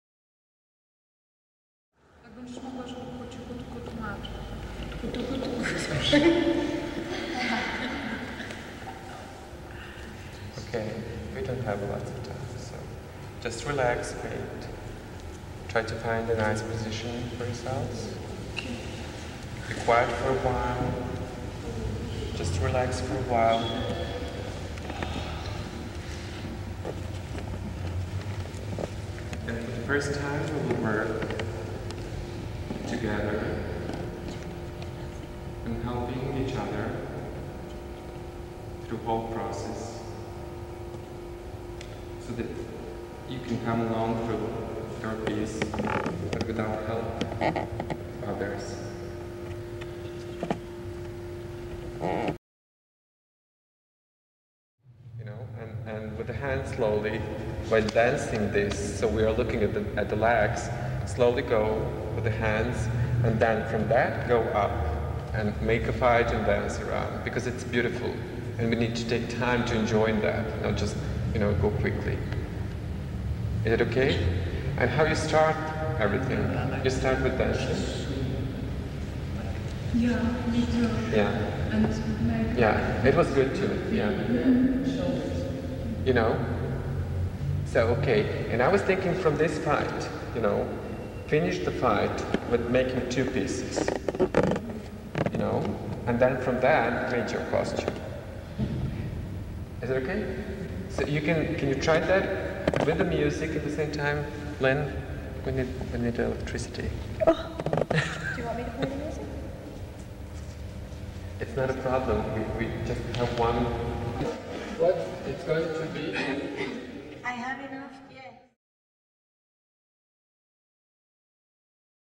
Project "5", 1995 - Bytom, Poland
International Project "5-Five" - author Boris Caksiran
(Yugoslavia, USA, Poland, Columbia)
Five dancers research into the use of costumes - presented on the main stage of the festival - excerpts from the rehearsal.
(1996) Annual International Contemporary Dance Conference and Performance Festival, Bytom, Poland